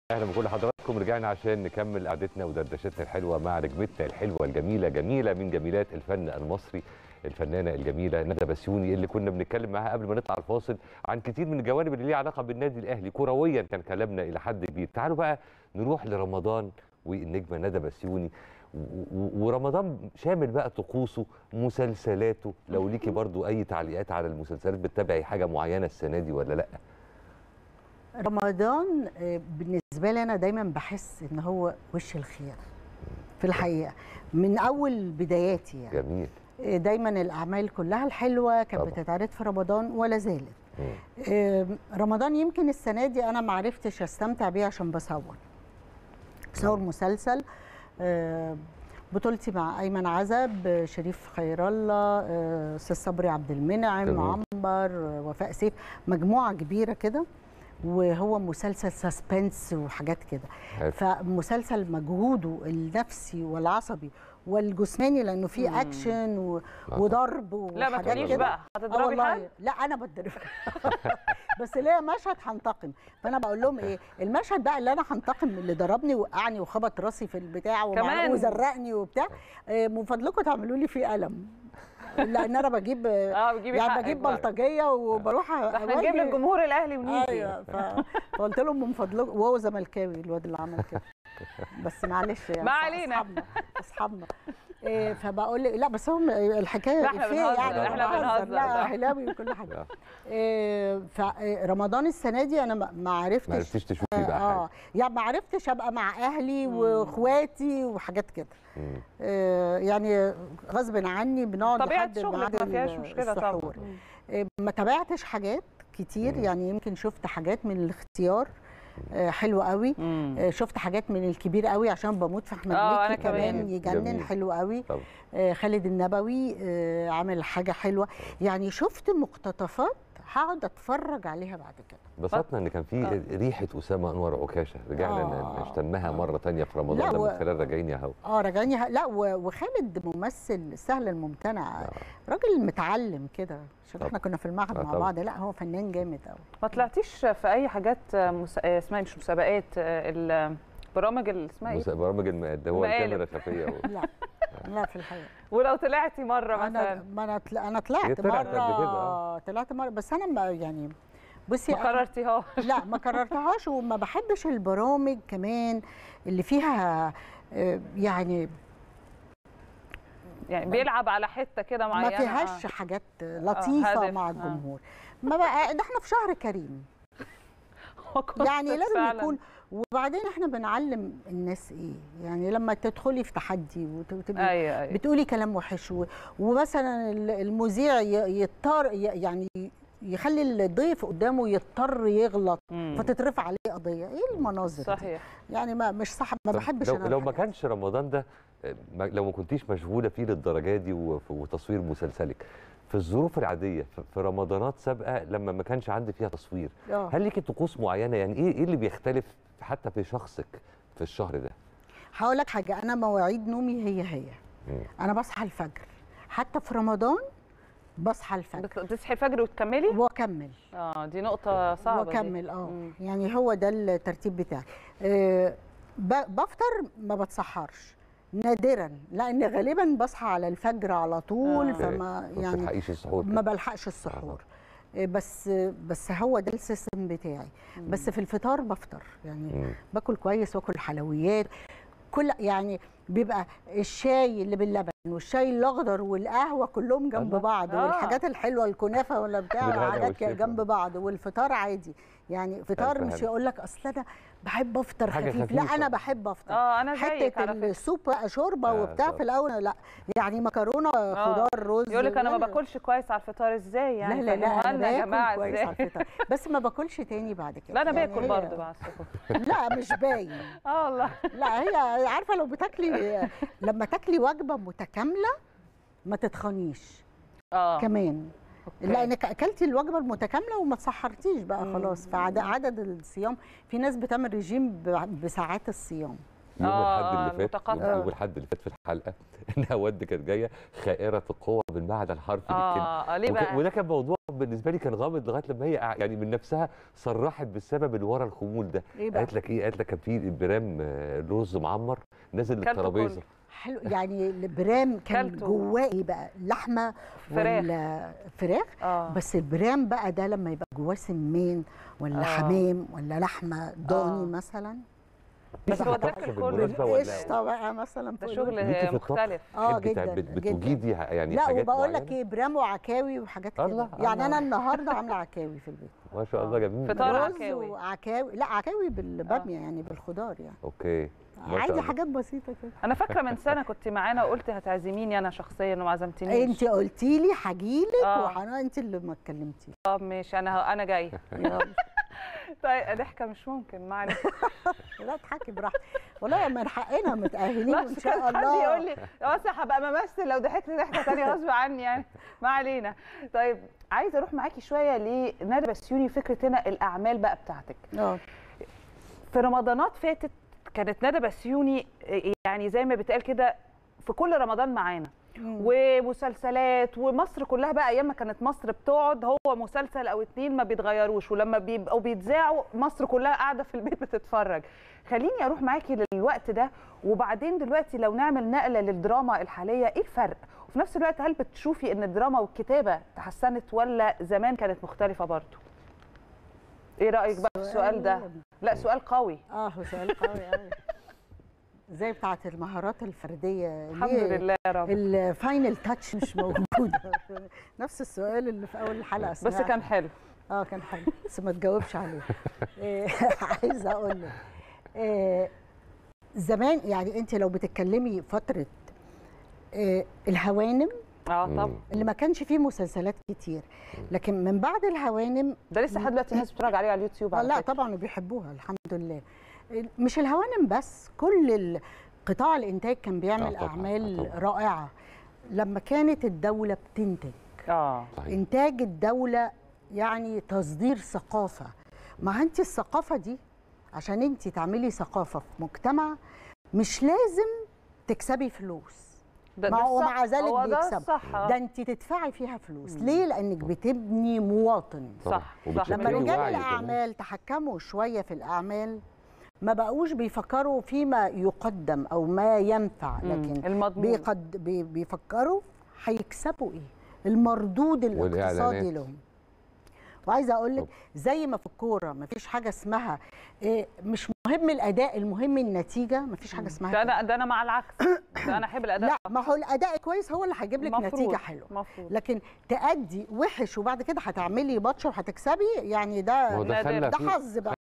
الفنانة ندى بسيوني تتحدث عن طقوسها الرمضانية ما بين الأعمال الدرامية والمباريات الكروية.
الفنانة ندى بسيوني تتحدث عن طقوسها الرمضانية ما بين الأعمال الدرامية والمباريات الكروية | الأهلي في رمضان